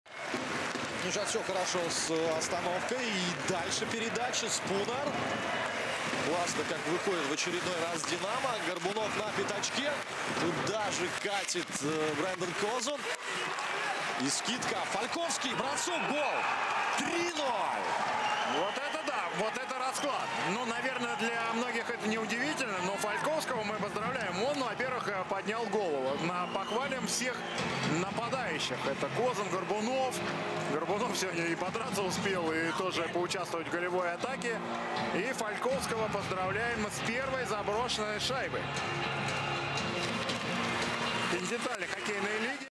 Falkovsky first KHL goal
Stepan Falkovsky scores his first KHL goal

Степан Фальковский открывает счет голам в лиге